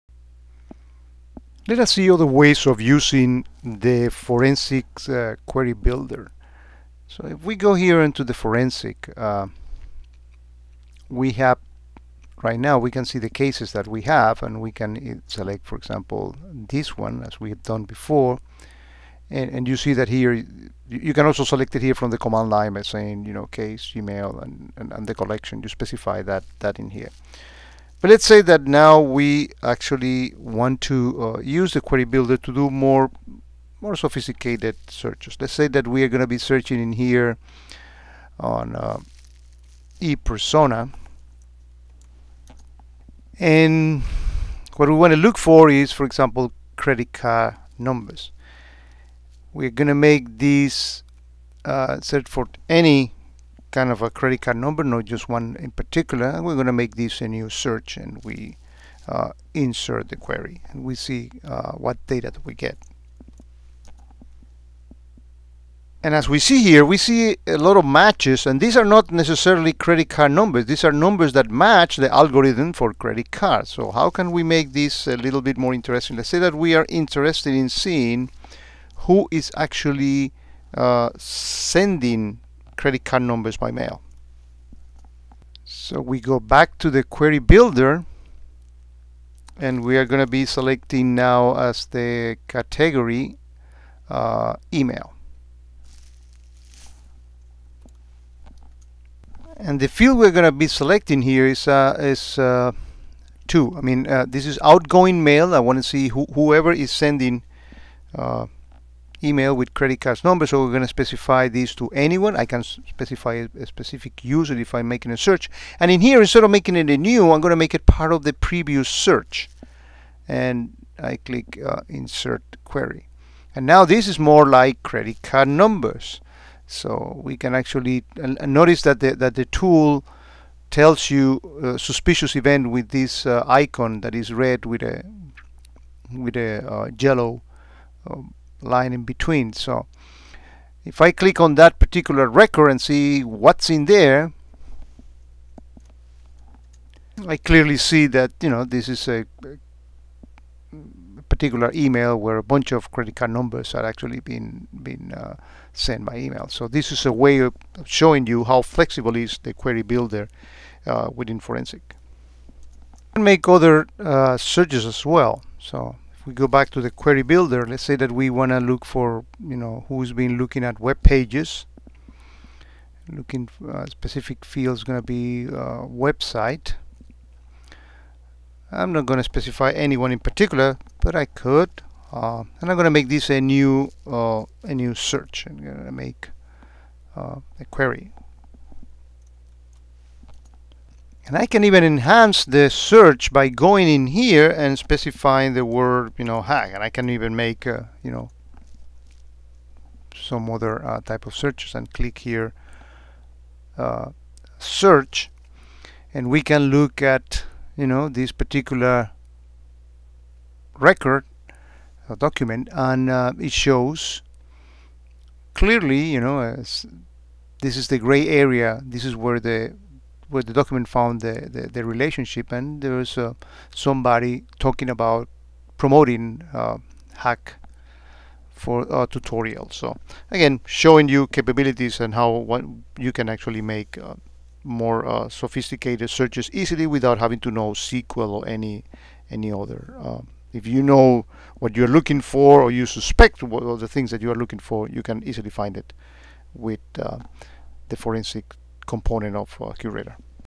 Forensics Case 4
You do not need to know SQL to make sophisticated searches using QRadar Forensics. In this video we show some of the capabilities of the Query Builder to filter data.